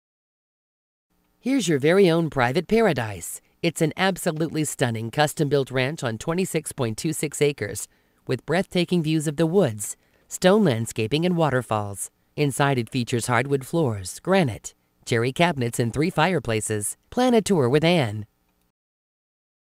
Home for sale in Bethel, OH | $464,900
Your very own private paradise-Absolutely stunning Custom built Ranch on 26.26 acres-beathtaking views of the woods and stone/rock landscaping-waterfalls-Hrdwood floors,Granite,cherry cabinets-3-FP-inground pool-walking trails-